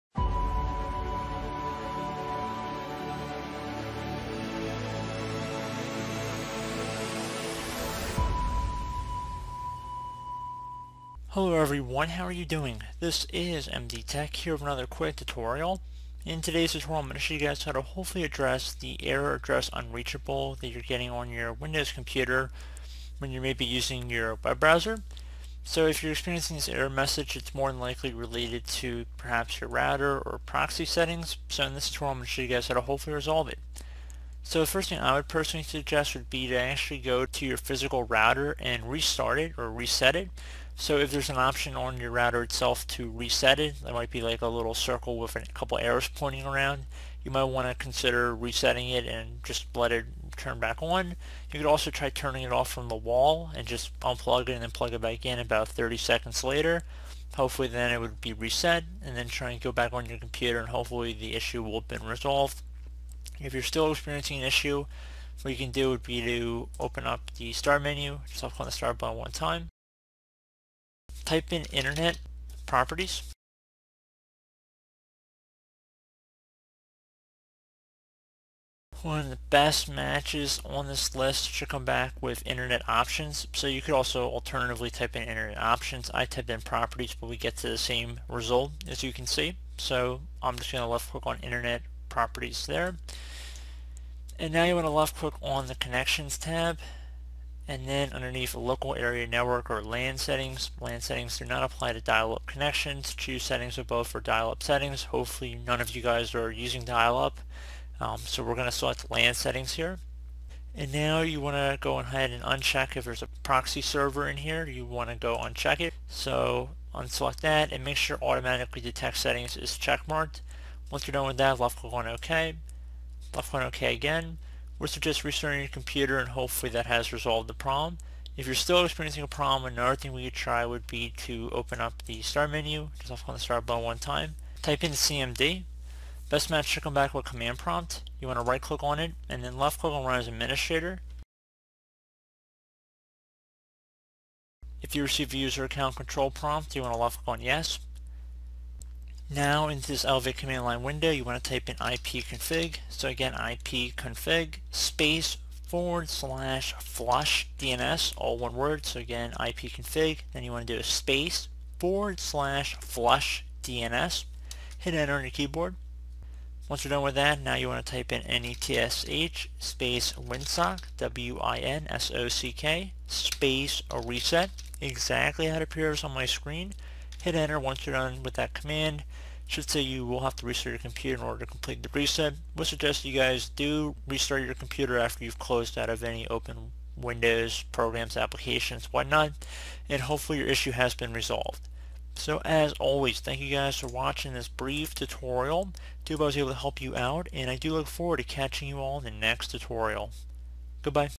Fix ERR_ADDRESS_UNREACHABLE in Google Chrome in Windows 10 [Quick Fix]
Users experience the error message

“ERR_ADDRESS_UNREACHABLE” when they are unable to access certain websites on their computer. It should be mentioned beforehand that this issue can be either at your side or at the server side.

If the problem is at your side, the router or your internet settings are mostly in fault. If the issue is on the server’s side, the server might not be accepting the requests on the required port addresses. This is a very generic issue and if the issue is at the client’s side, it is mostly fixed instantly.